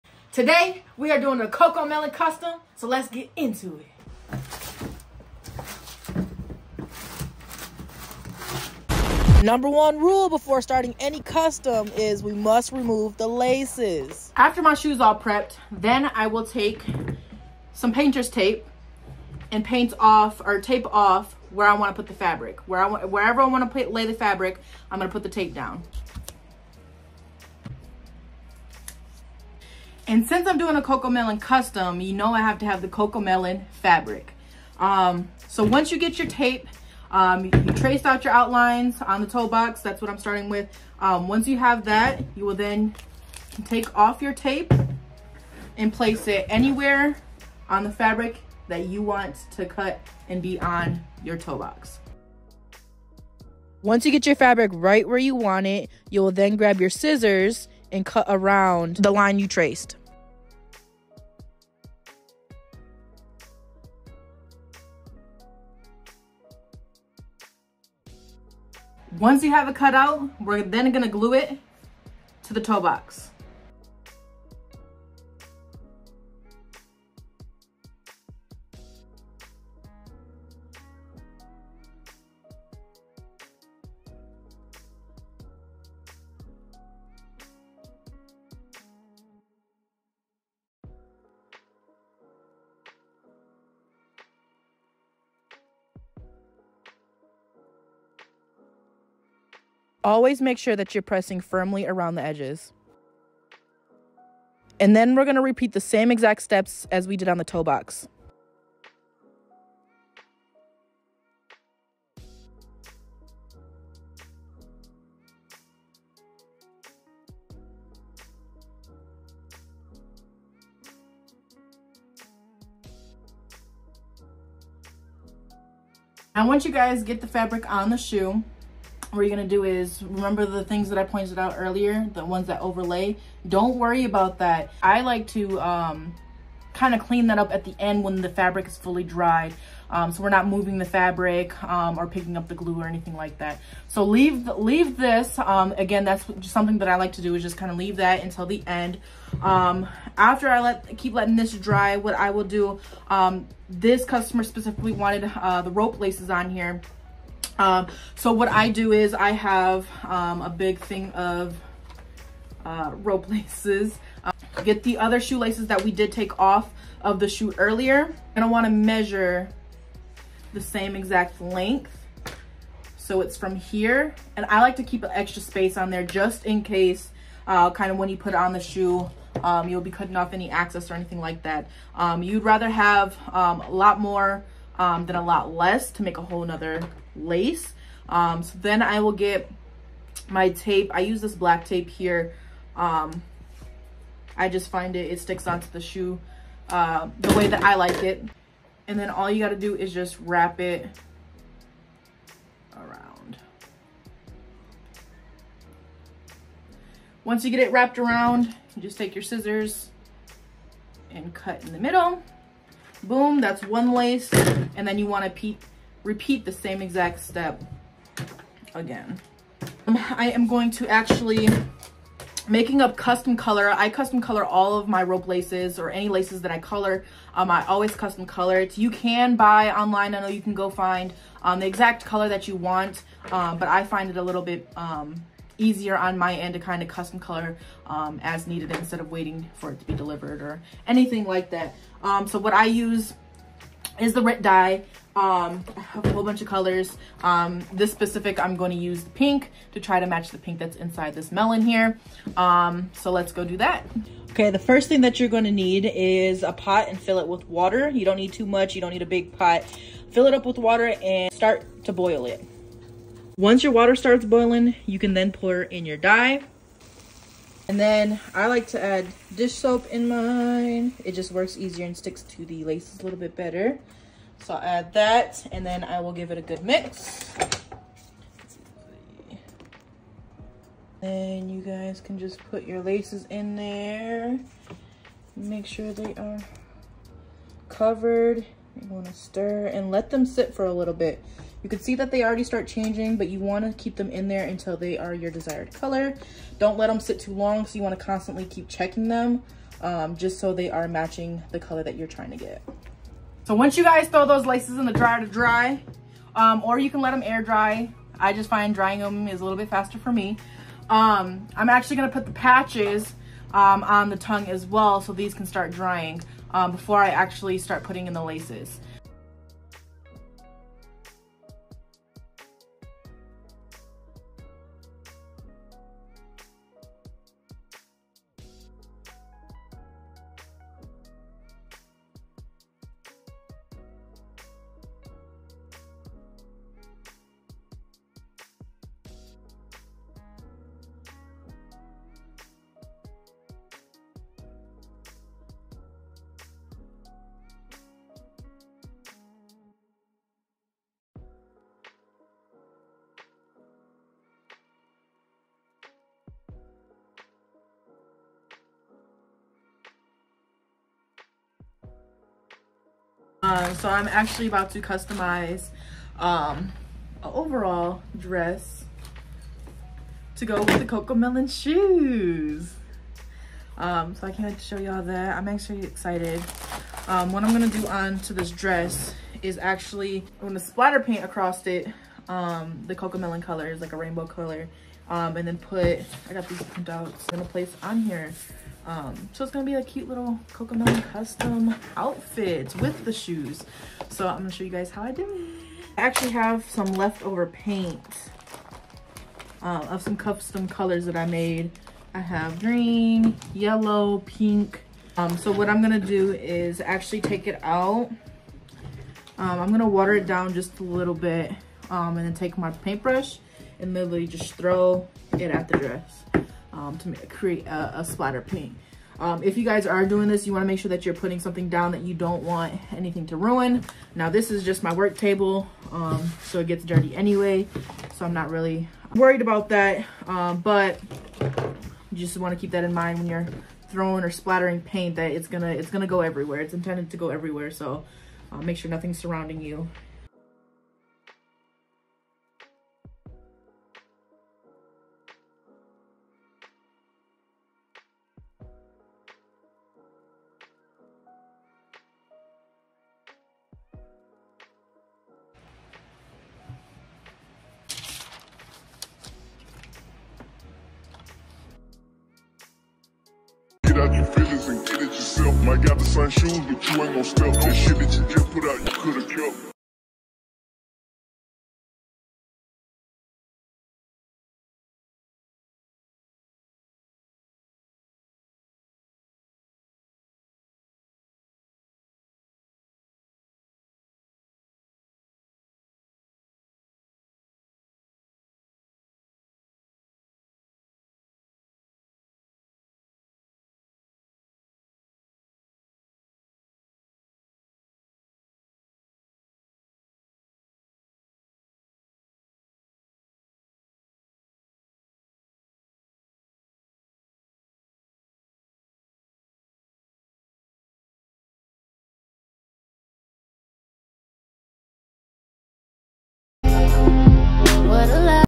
HOW TO: Add patches to AirForce Ones
Want to learn how to add the patches to the tongue of your air forces??? Here’s how….